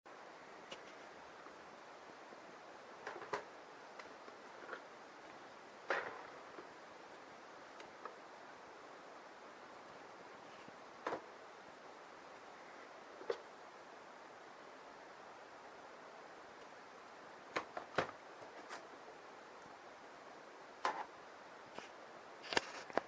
What to do with old DVD covers? DVD storageboxes DIY tip!
old dvd old cd storage diy recycle tip